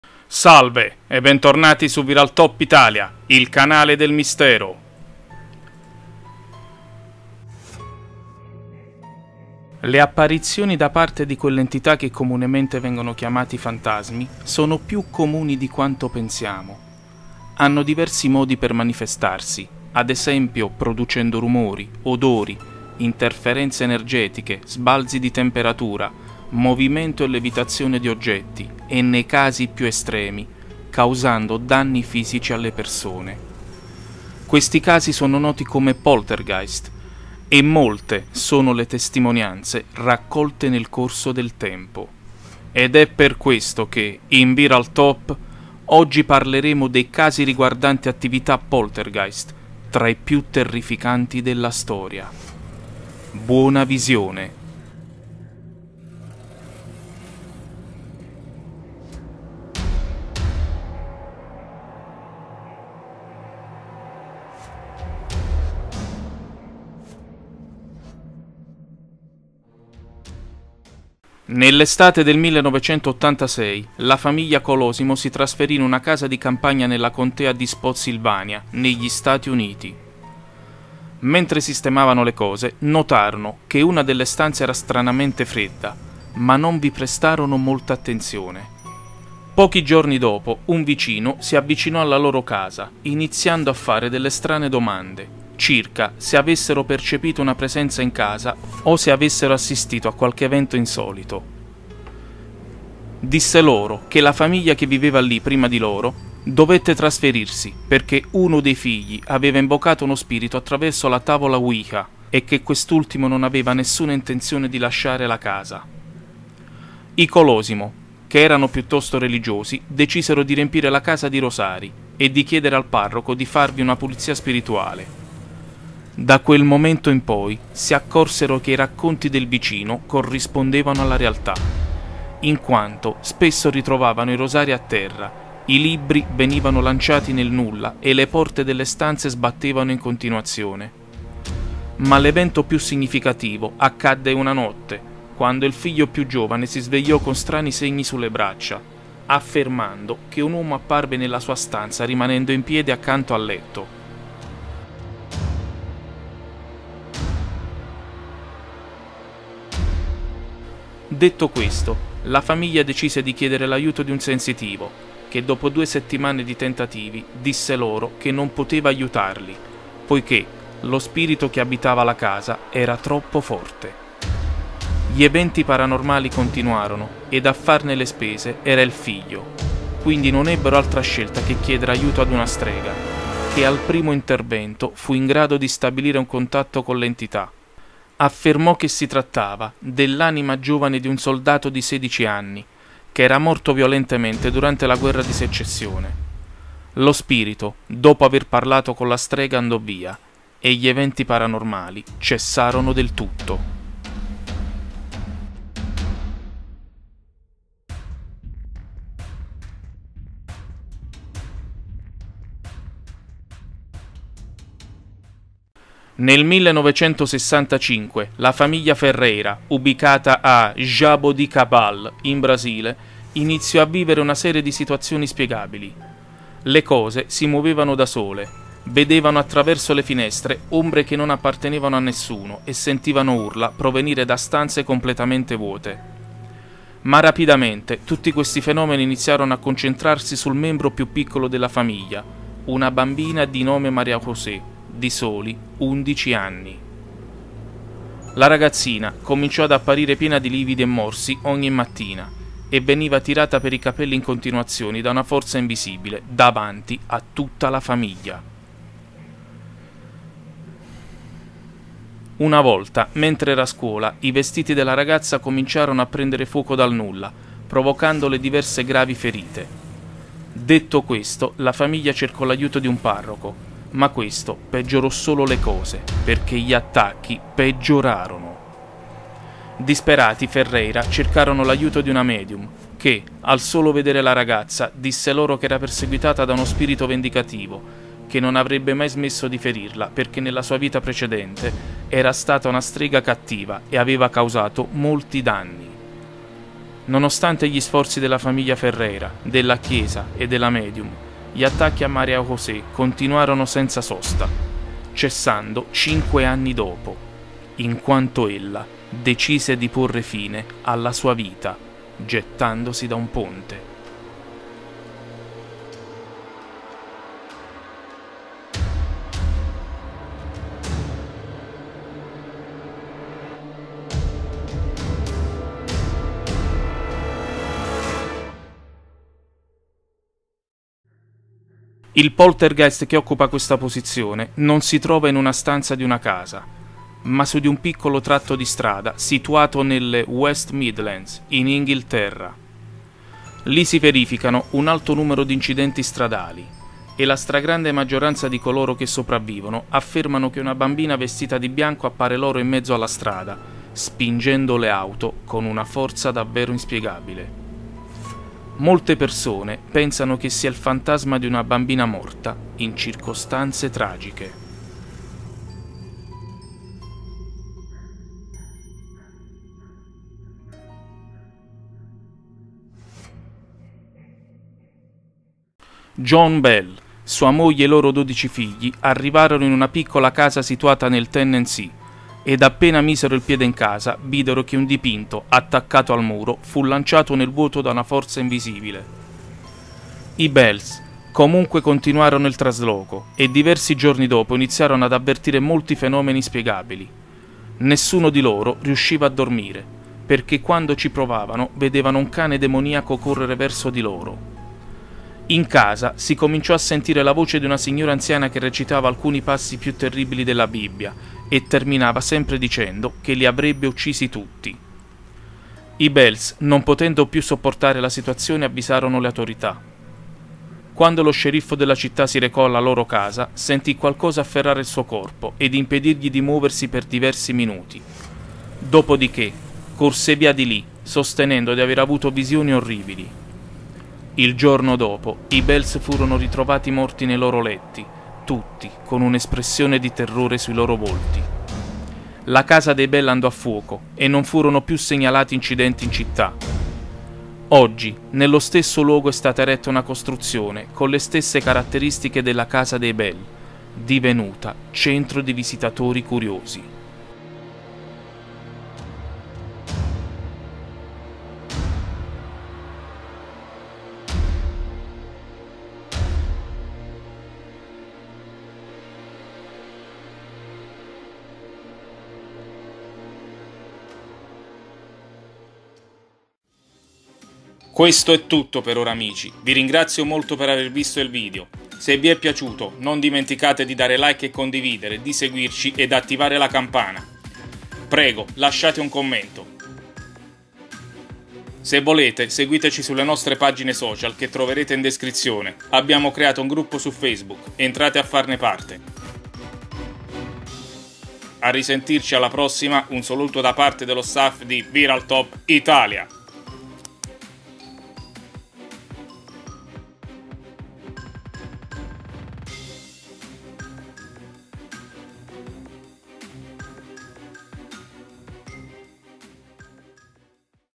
I casi di Poltergeist più terrificanti della storia
Come poltergeist, viene chiamato il fenomeno paranormale che consiste nell'apparente movimento, spostamento o levitazione di oggetti, nonché nell'irruzione di colpi, suoni e altri eventi inspiegabili, che causano anche danni fisici alle persone. Nel corso della storia, nel mondo si sono verificati molti fenomeni di questo tipo ed è per questo che ti presenteremo I casi di Poltergeist più terrificanti della storia

Speriamo che vi piaccia!